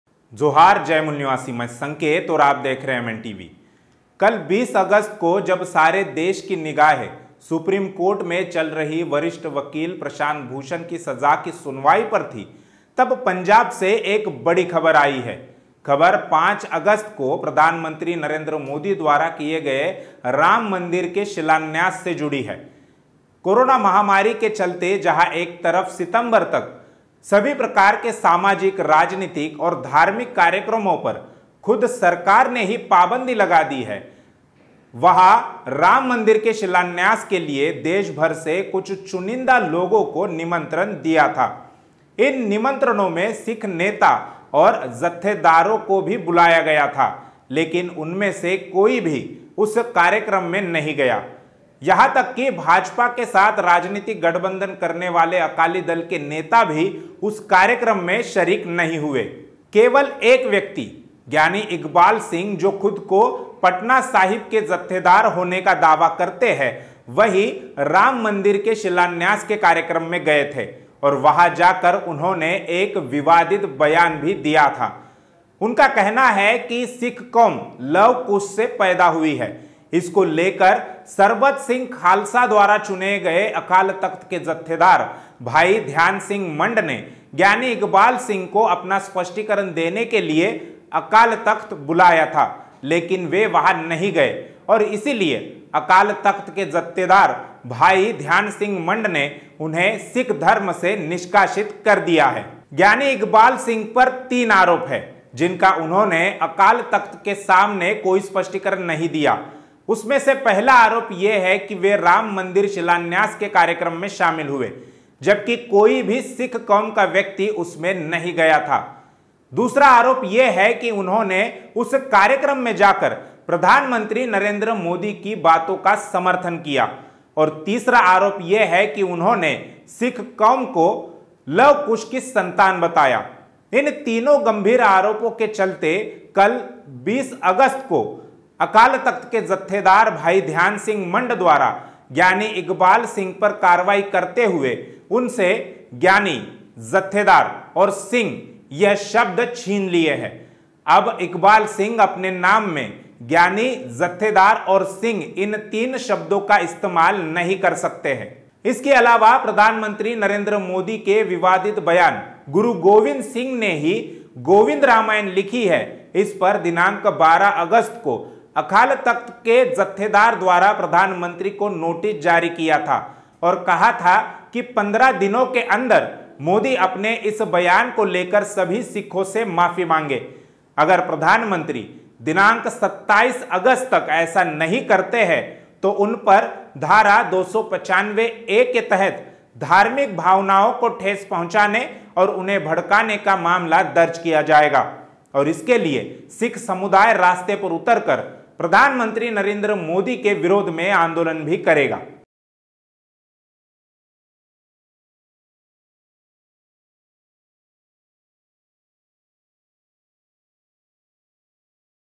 Sikhs Rage on PM Modi, Giani Iqbal Singh is Dispelled with Religion, Case May Be Filed Against Modi
4. MN Radio (android app)
5. Mulnivasi Nayak (android app)
6. MN News (Web Portal)
7. MN News (Web Portal)